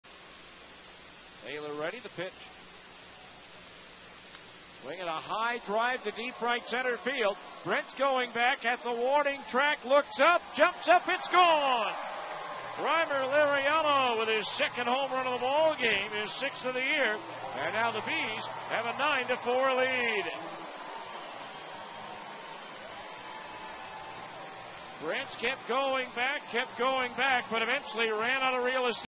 Rymer Liriano homers twice
5/5/18: Rymer Liriano homers for the second time to help Salt Lake build a big lead.